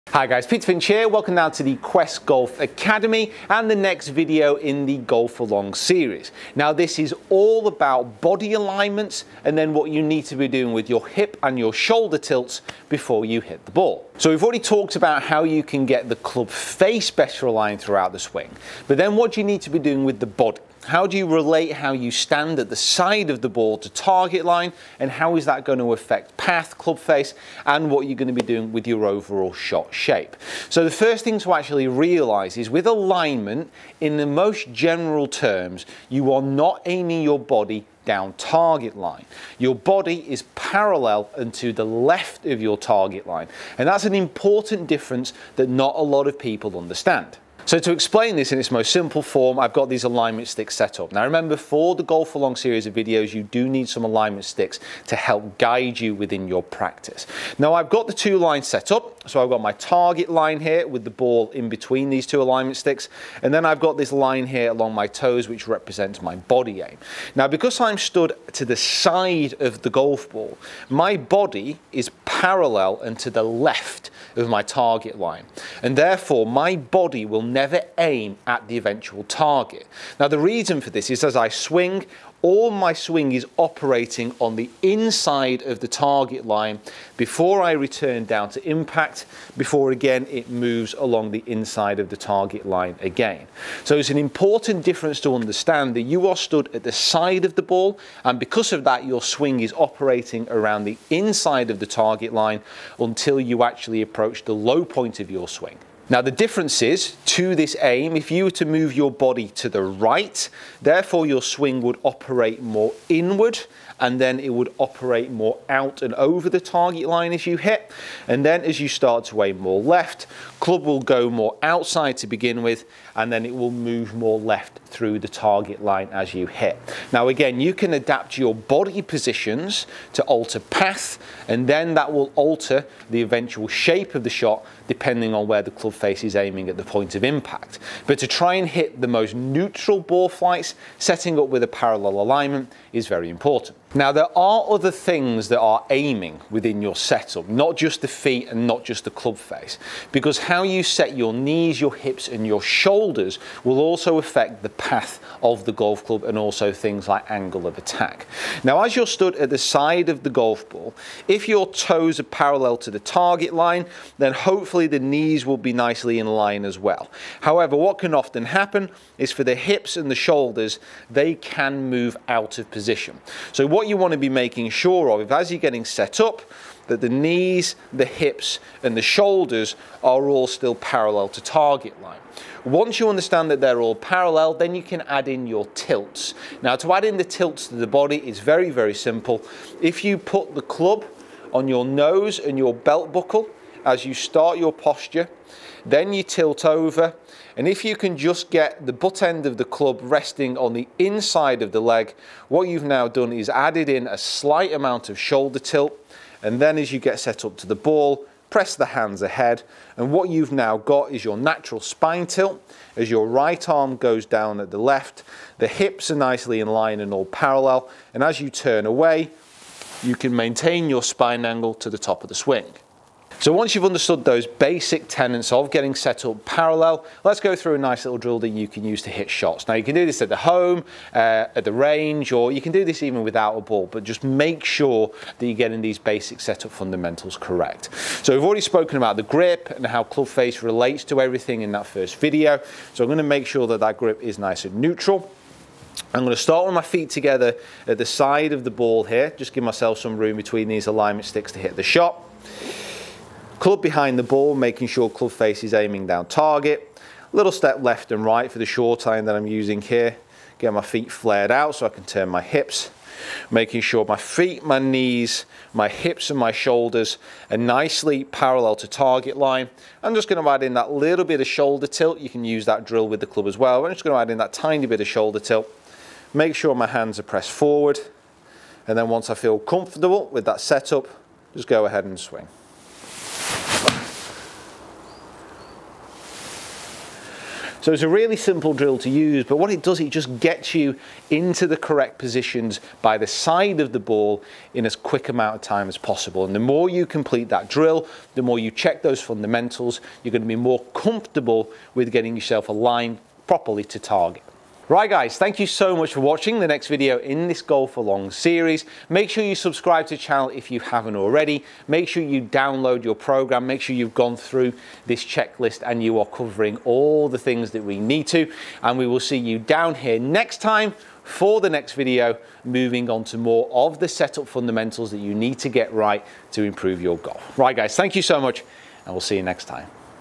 How To Aim The Body In Golf
GolfAlong Video Three - How to aim and align the body in golf. In the next GolfAlong video we look at how golfers can better aim themselves